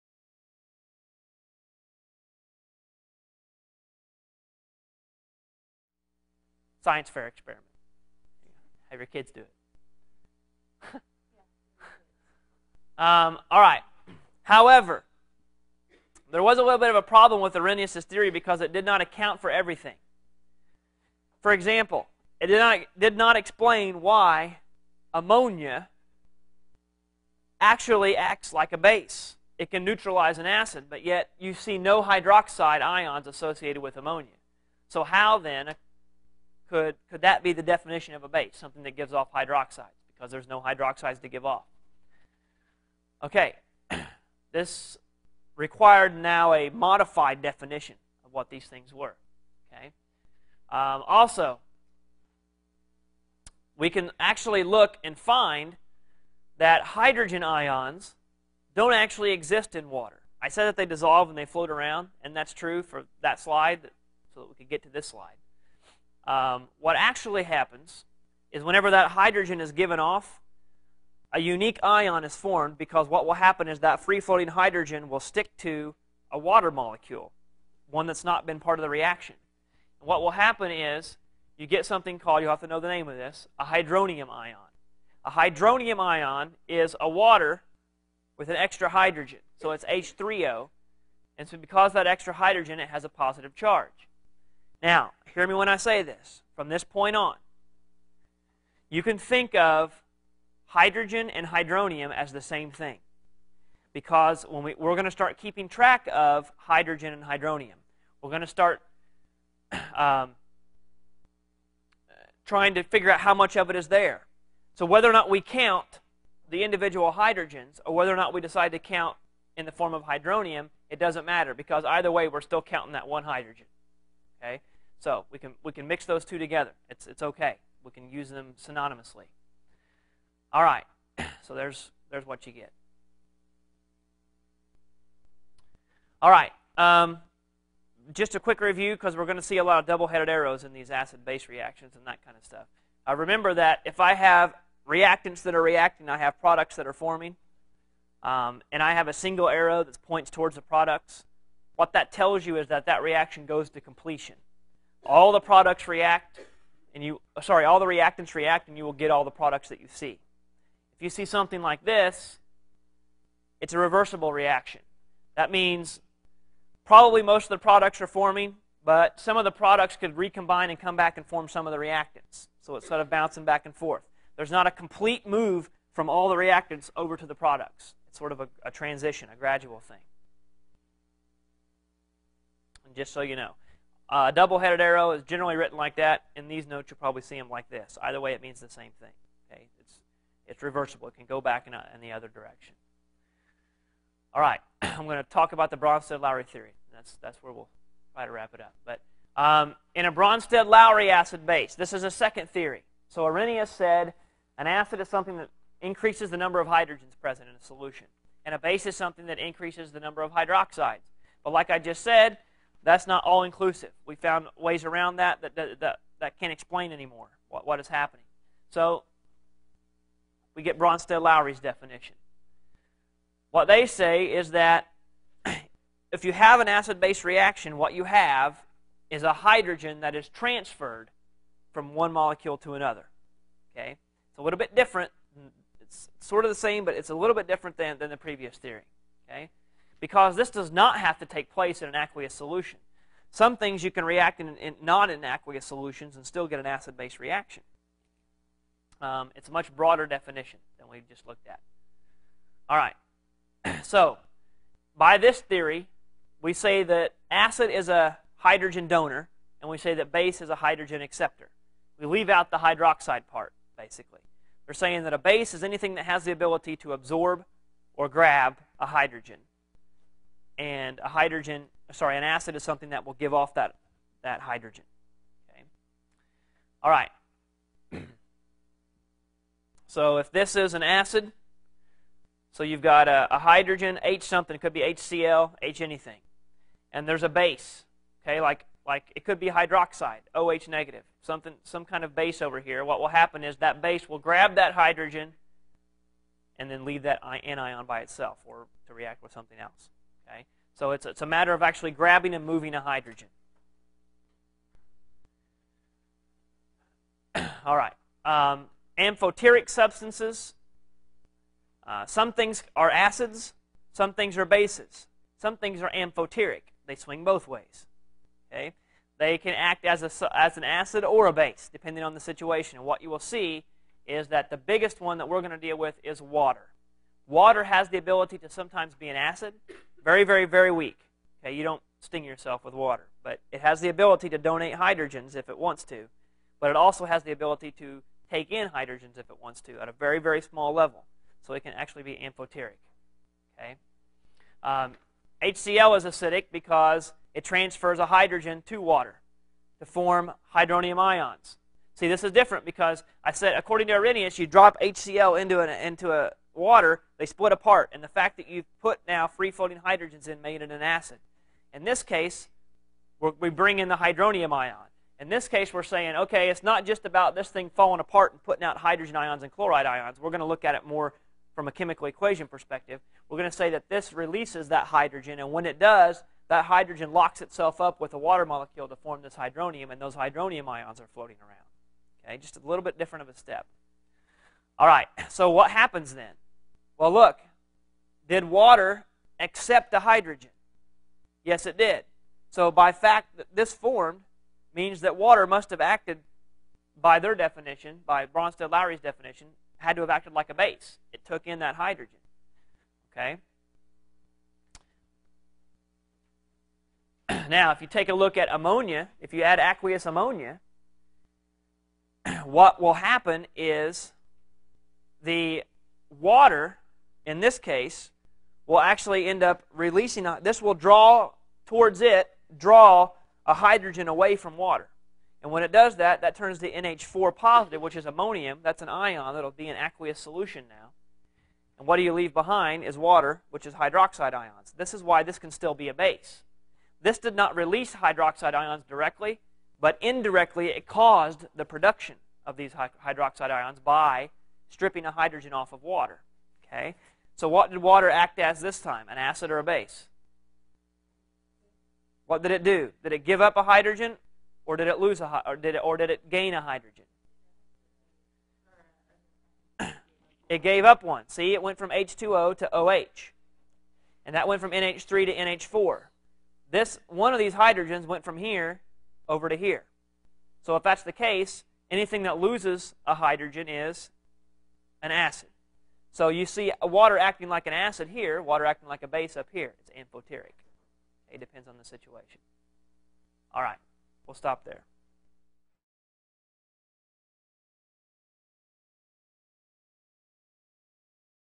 Chapter 14 Acids and Bases Part 4 of 19
Jim Brinson Associate Professor of  Physical and Life Sciences at Ivy Tech Community College discuss Chapter 14 Acids and Bases. In the fourth video of a 19 part series.